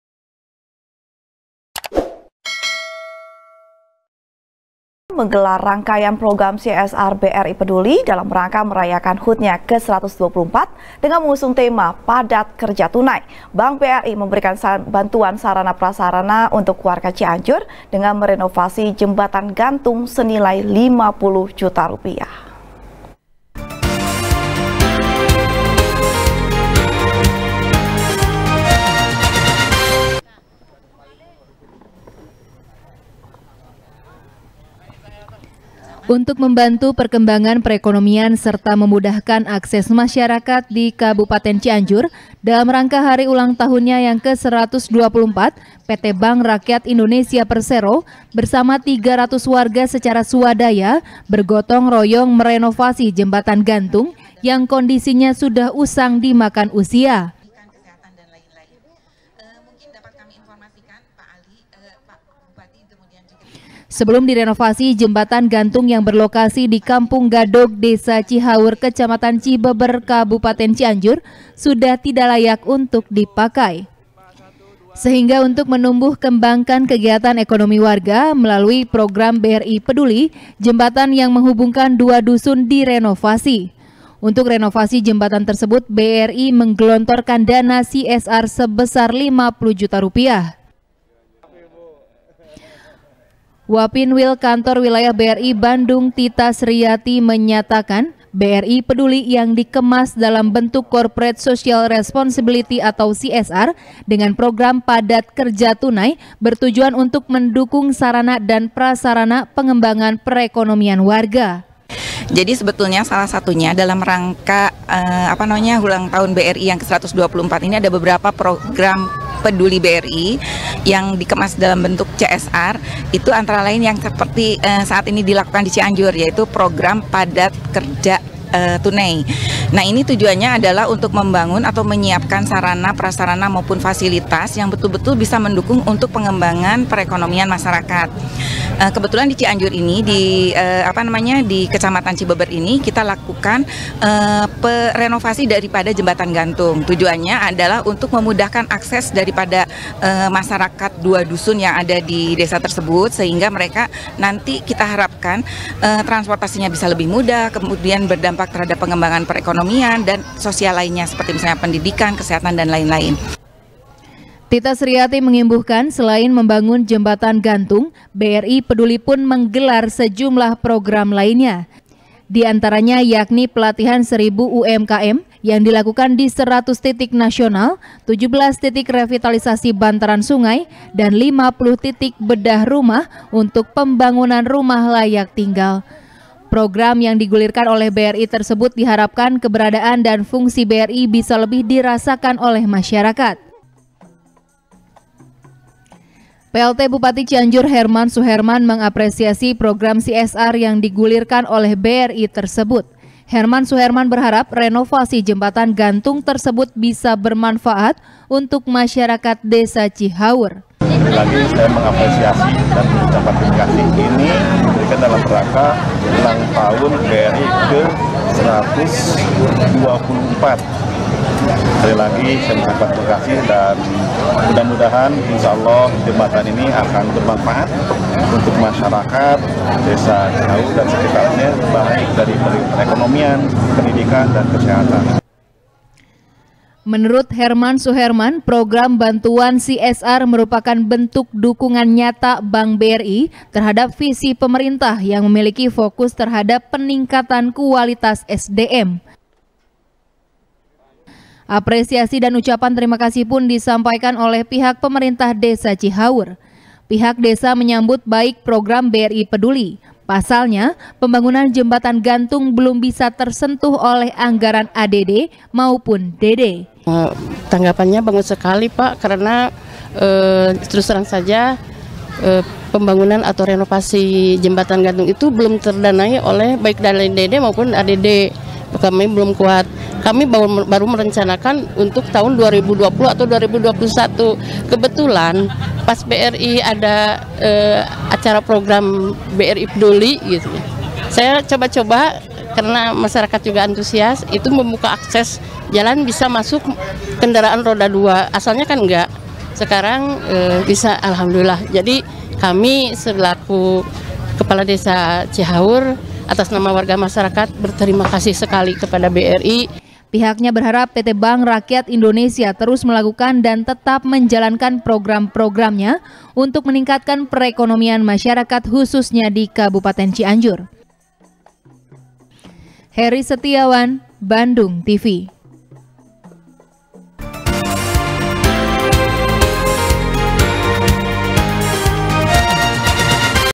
Rayakan HUT Ke124 BRI Renovasi Jembatan Gantung Untuk Warga | SBR | BANDUNGTV
SBR | FARHAT MUMTAZ | Empay Suryati.S,E  Kepala Desa Cihaur
PT Bank Rakyat Indonesia (Persero) TBK Menggelar Rangkaian Program CSR BRI Peduli,Dalam Rangka Merayakan HUT Nya Ke-124,Dengan Mengusung Tema Padat Kerja Tunai. Bank BRI Memberikan Bantuan Sarana Prasarana Untuk Warga Cianjur,Dengan Merenovasi Jembatan Gantung Senilai 50 Juta Rupiah